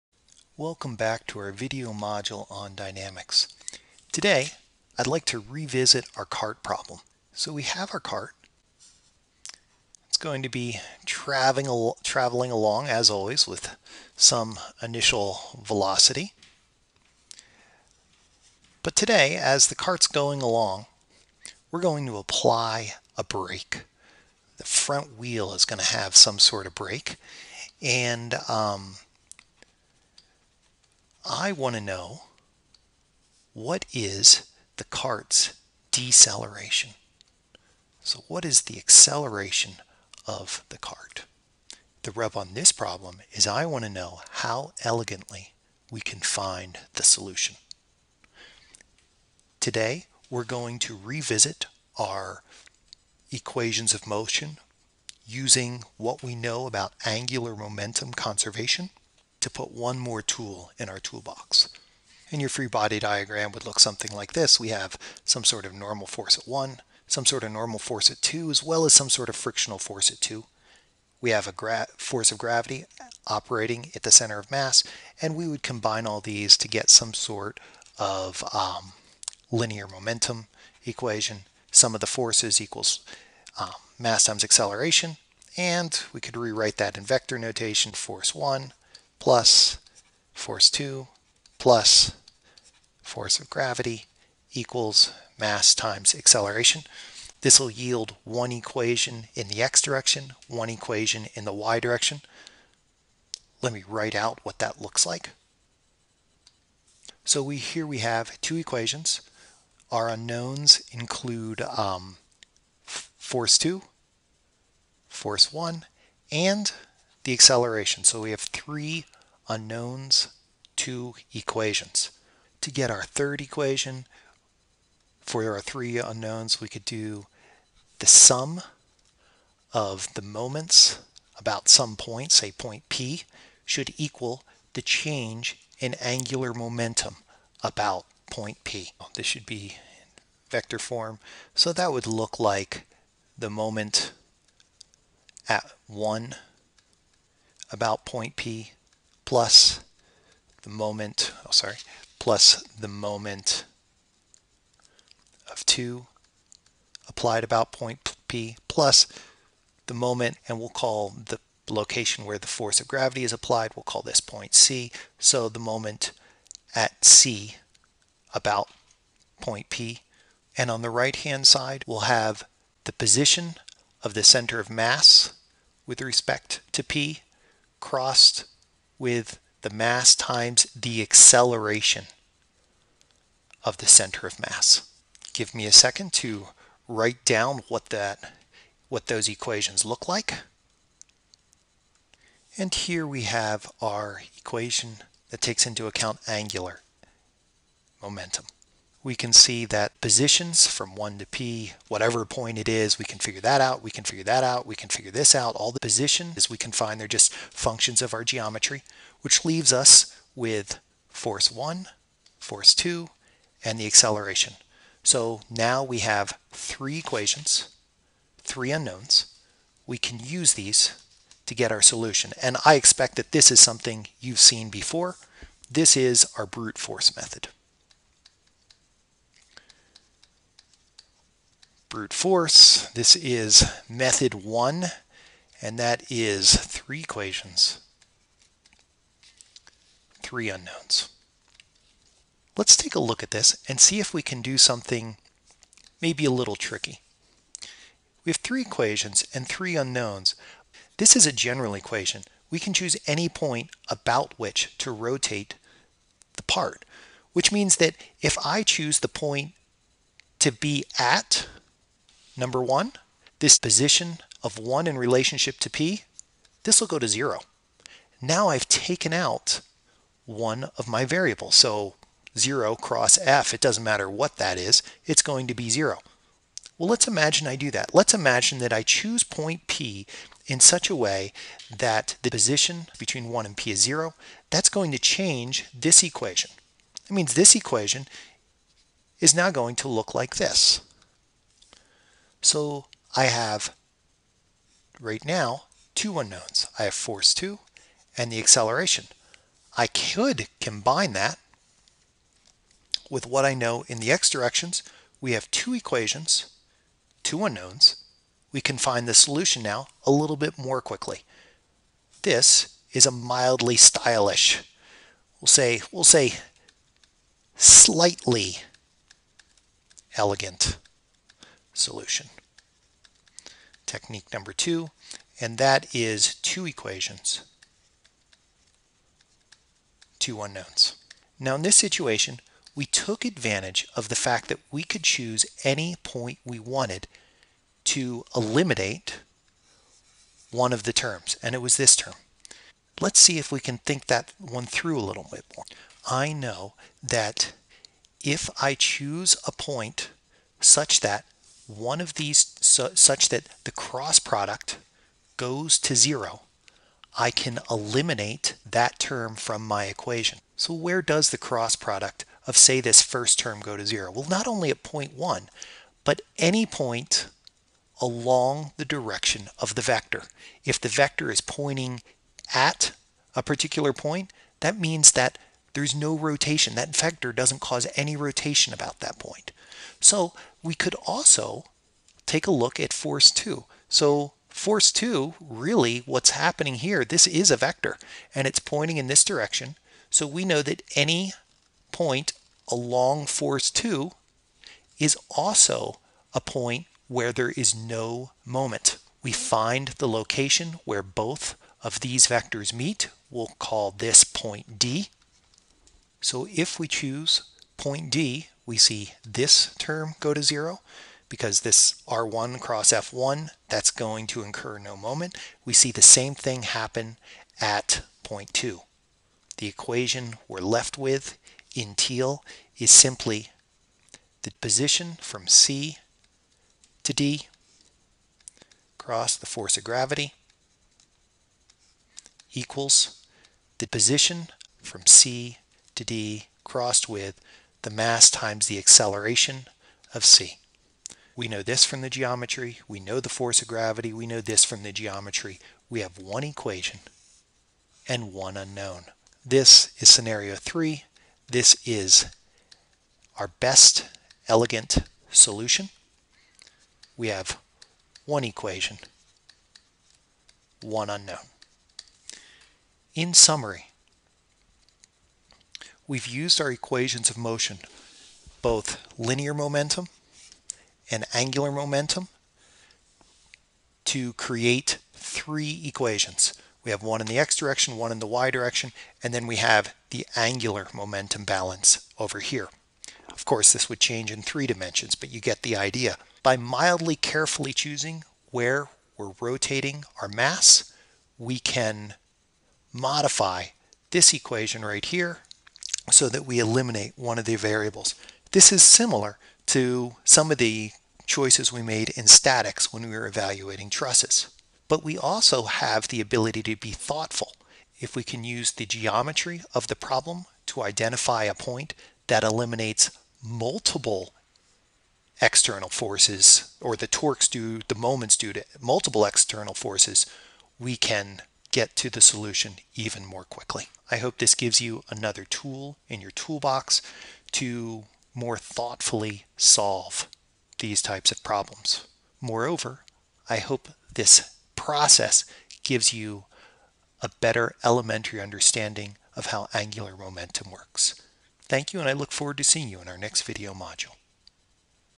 Engineering Dynamics 11.2-03 Elegant use of angular momentum
This video module explores different ways to use angular momentum to analyze motion.  Particularly, we look at the brute force method of using 3 eqns and 3 ?, then a slightly elegant technique of choosing our rotation point to get 2 eons. and 2?  Finally, we choose our rotation point elegantly to get 1 eqn. and 1?